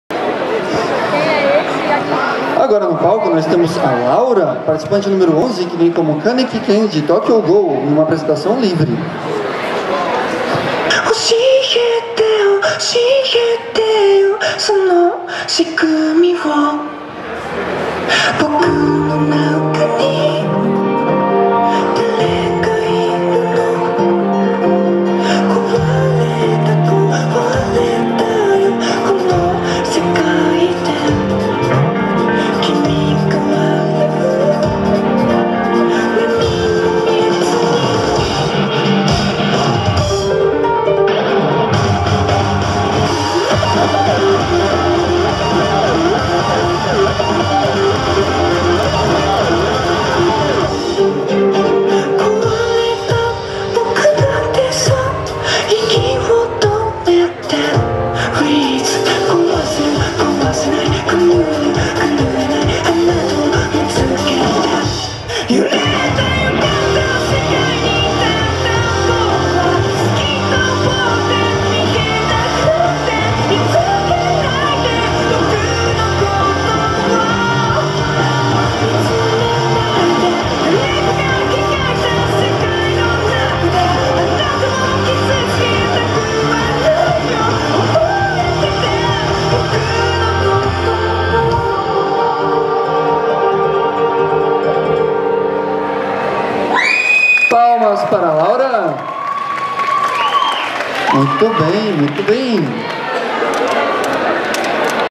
AniQuest 2015 - Concurso de cosplay: Kaneki
Cosplay de Ken Kaneki (Tokyo Ghoul) em apresentação livre no concurso de cosplay do AniQuest 2015.